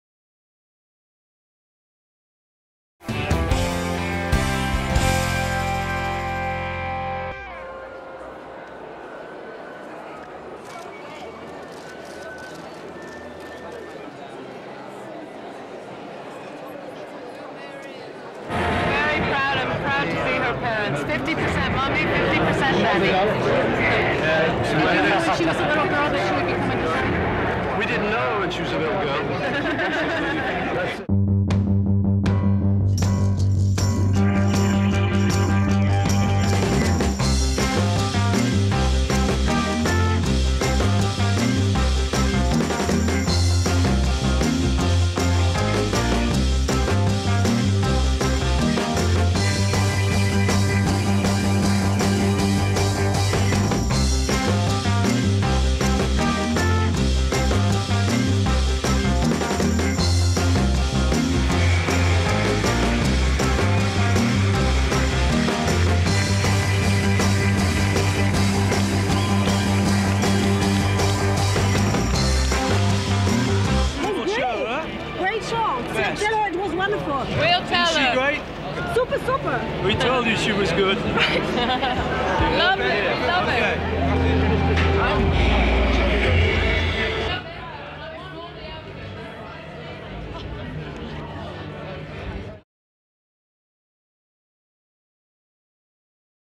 On This Day: 15 October 1997
British designer Stella McCartney made her Paris Fashion Week debut for Chloe. (Oct. 15)